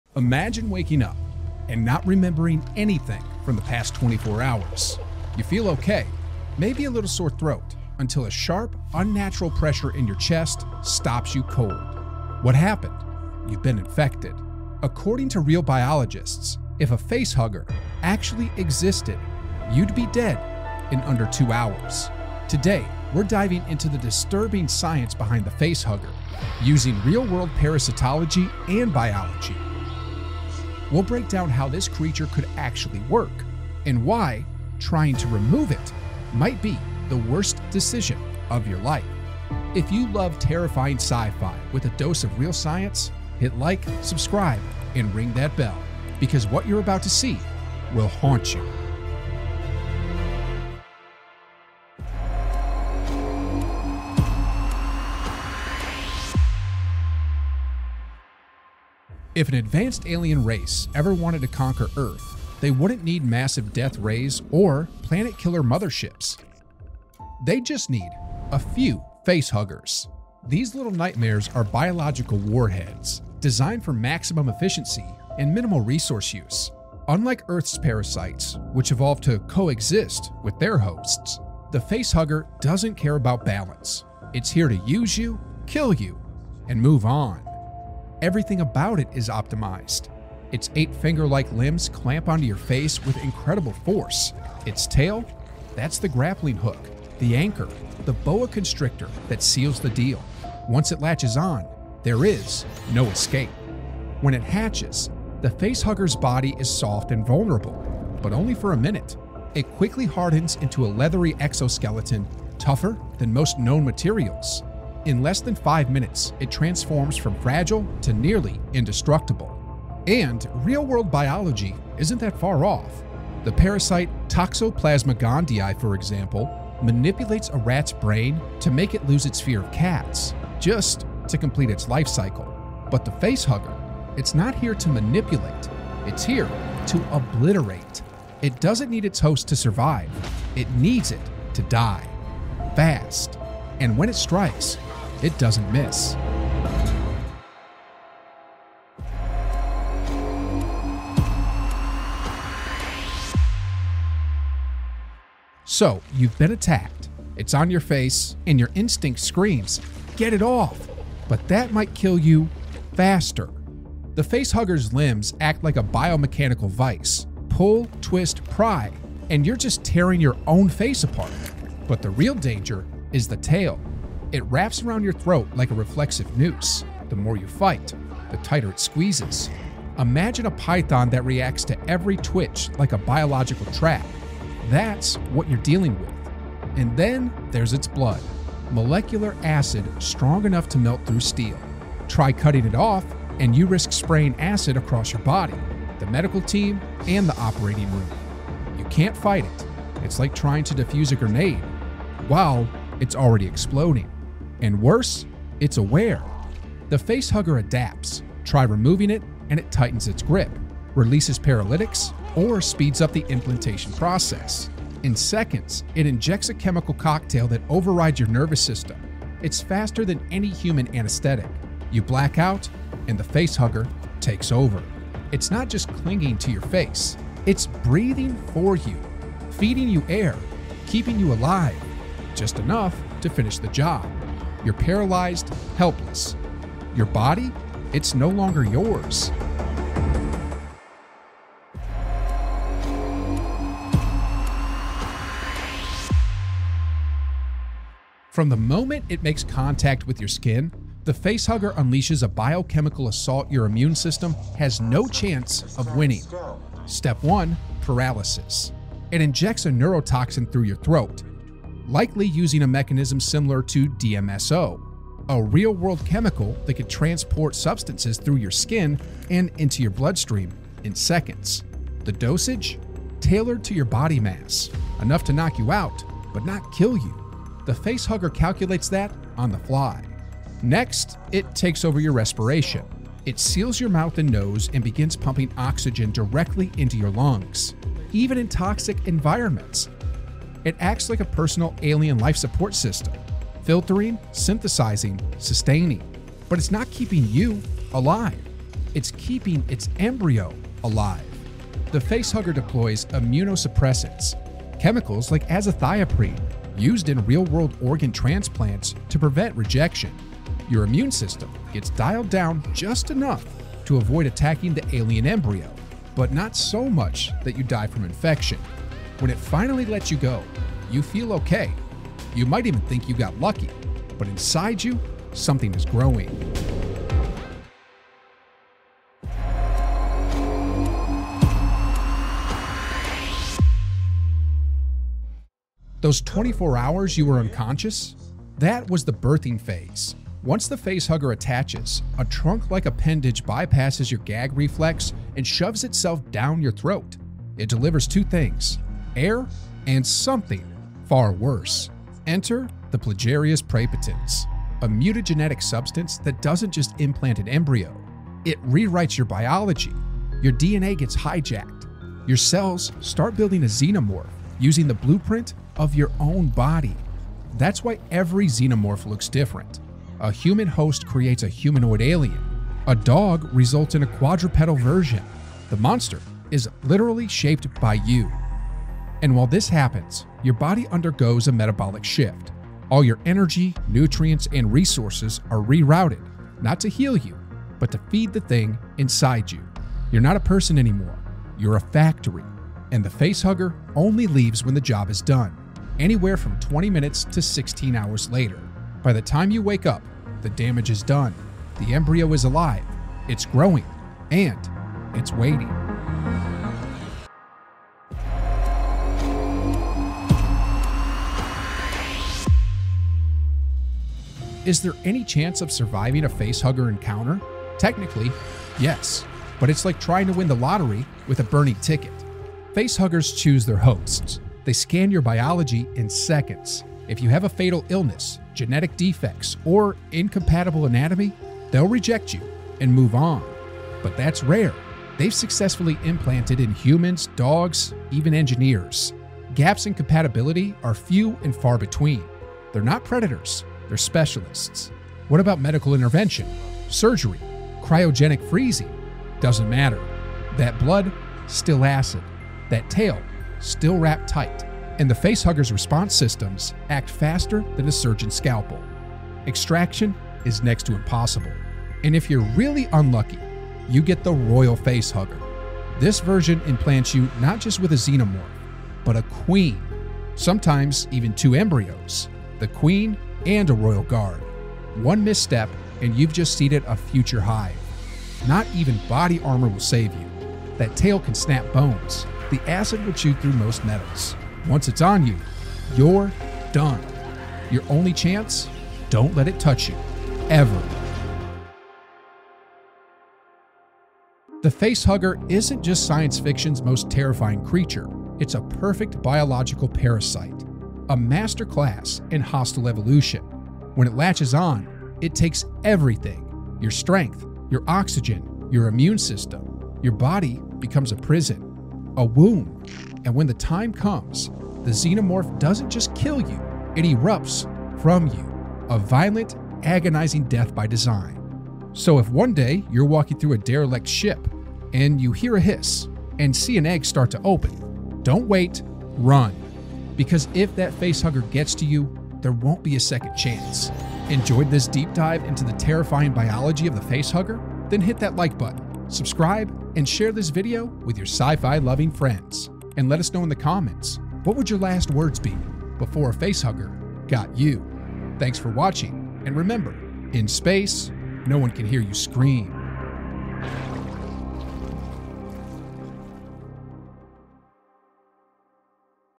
How FACEHUGGERS Impregnate Hosts But Also Keep Them Alive?
From the twisted world of Alien and Aliens vs Predator, the Facehugger isn’t just a sci-fi monster—it’s the ultimate biological weapon. But what if it could actually exist?

In this video, we dive deep into the disturbing science behind how a Facehugger really works. From real-life parasites like Toxoplasma gondii to terrifying chemical warfare inside your body, discover how this nightmare creature could implant a Xenomorph embryo—and why ripping it off your face would almost certainly kill you faster.

Chapters:
00:00 Introduction
00:56 The Perfect Biological Weapon
02:33 Can You Remove a Facehugger?
04:06 How it Keeps YOU Sleeping?
05:33 How it Impregnates You?
06:53 Survival Odds